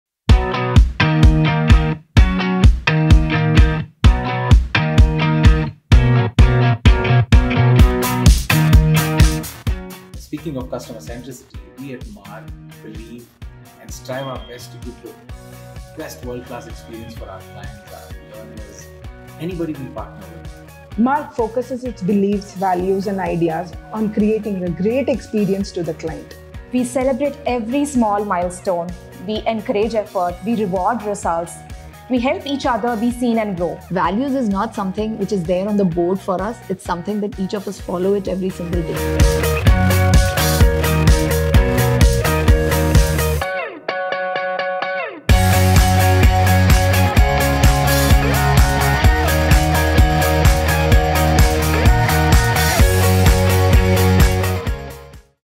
Team Celebrations At MARG
A year of shared triumphs, countless victories, and endless smiles! Join us in a grand celebration of unity and success as we revisit the cherished moments throughout the year.

Watch closely as each frame unfolds a story of unity, milestones achieved, and the unwavering joy that defines LifeAtMARG.

Cheers to the journey, the moments, and the laughter—captured for eternity in our MARG family album!